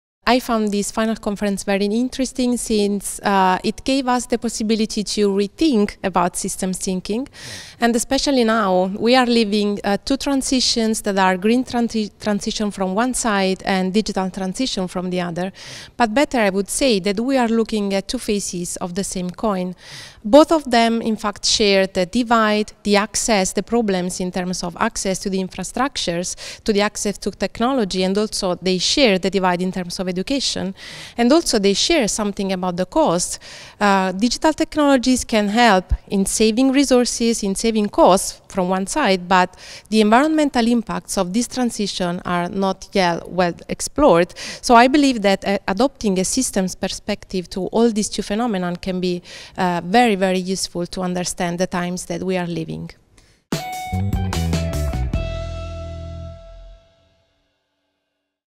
Francesca Iandolo (Sapienza Università di Roma) alla Conferenza finale del progetto europeo Systema
Festival dello sviluppo sostenibile 2023: "Conferenza finale del progetto europeo Systema".

Ai nostri microfoni Francesca Iandolo, Ricercatrice dipartimento di Management
, Sapienza Università di Roma

Di Elis Viettone, in collaborazione con Patrizia Montesanti. Grafiche e montaggio di Francesca Spinozzi.